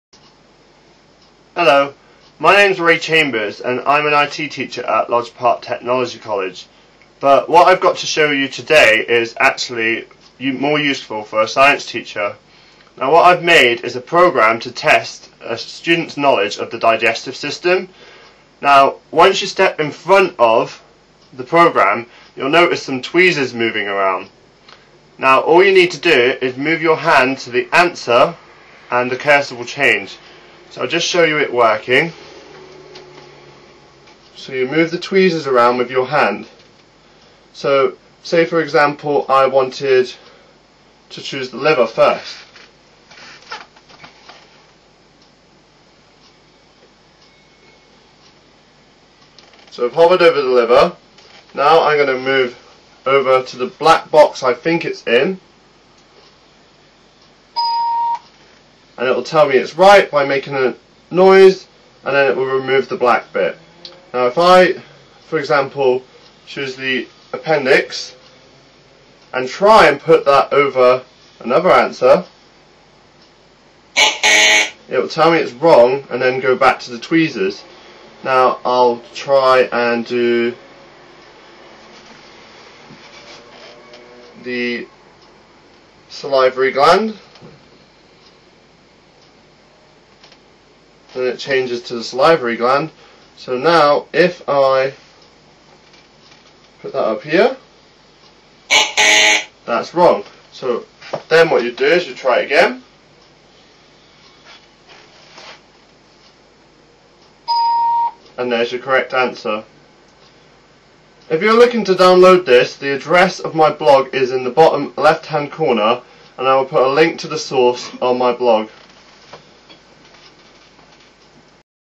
Digestive System (Kinect In Science)
A small program I made for use with the Kinect. It will test a students knowledge of the different parts of the digestive system. Hopefully it will help them retain the knowledge of where everything is. For more programs check out my blog - There are a few more on there.